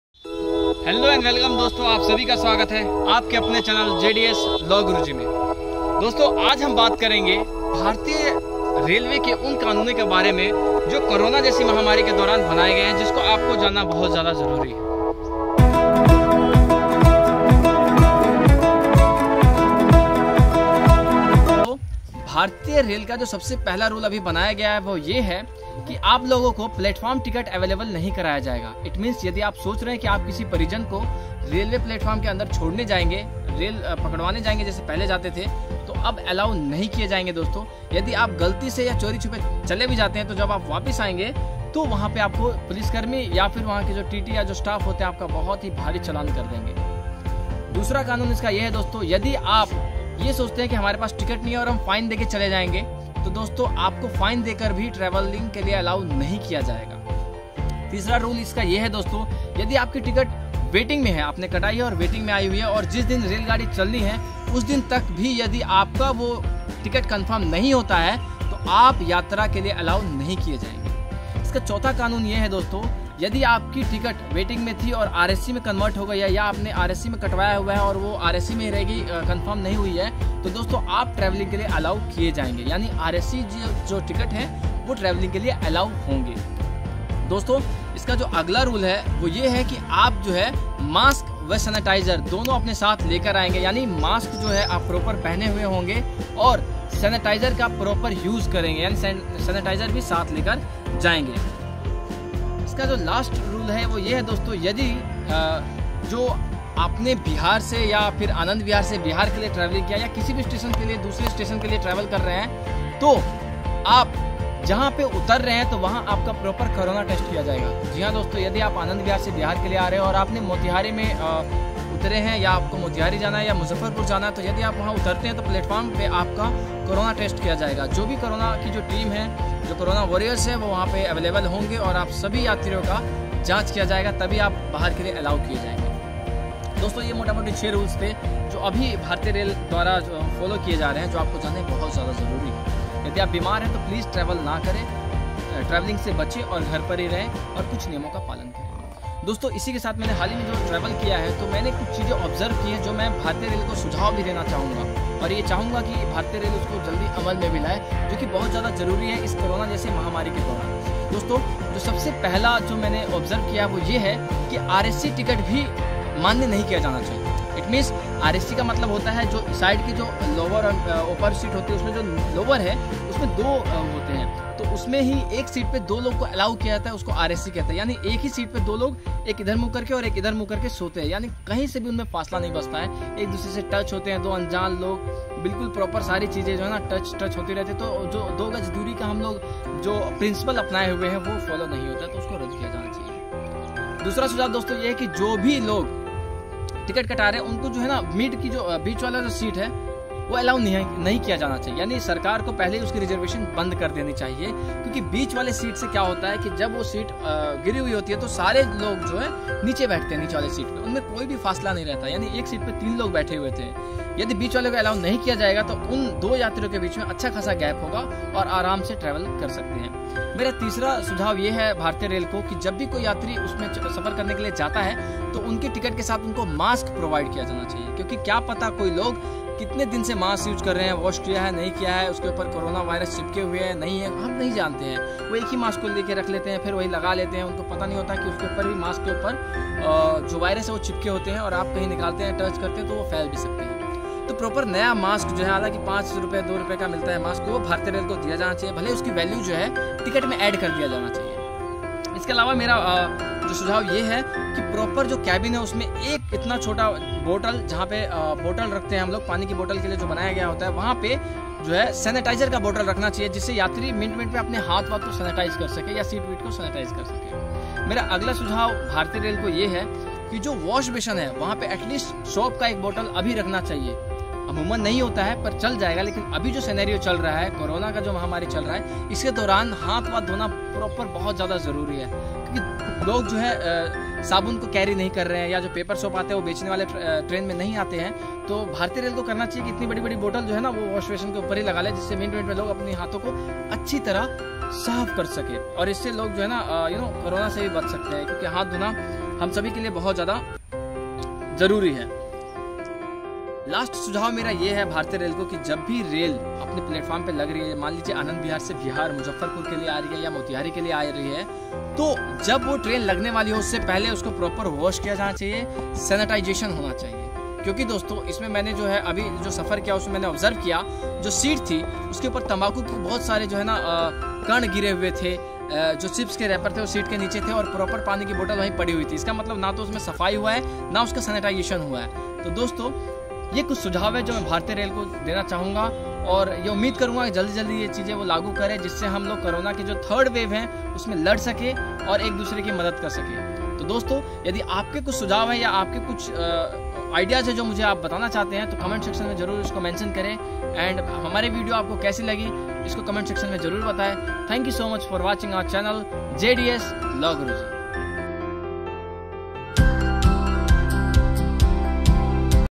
NEW RAILWAY RULES || CORONA VIRUS || ANAND VIHAR RAILWAY STATION || DELHI TO BIHAR || CORONA TEST
agar aap indian railway me travel kr rhe hai to in bato ka khash dhyan rakhe , ye hai new rules